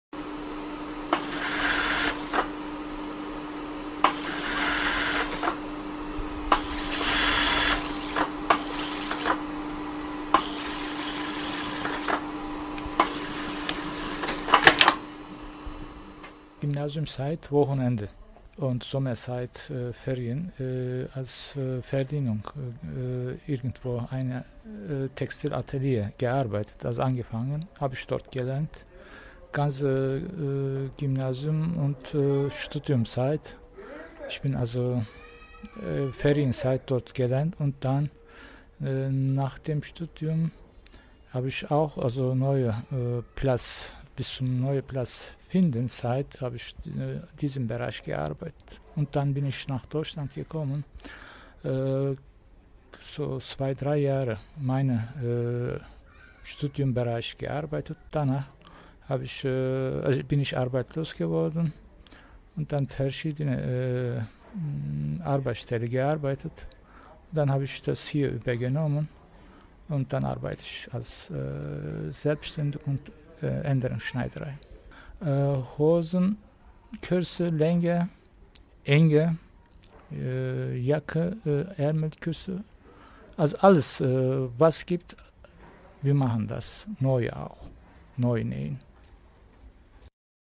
Türkischer Schneider
Many tailors in Germany have turkish roots like Mehmet Akif Arak in Hanau...

From an old series of multi-media reportages from 2012 and 2013, when nobody dared to use HD videos for the iPad edition of a newspaper.